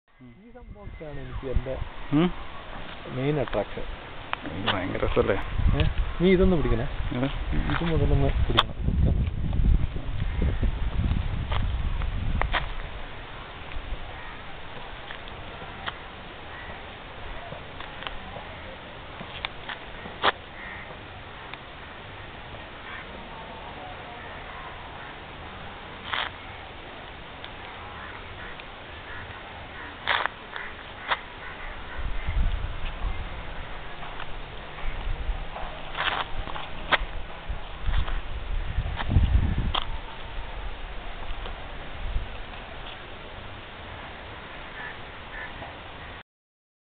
river kerala
Paddy fields in Kerala. ... According to the State Planning Board, Kerala lost over 500,000 hectares of paddy fields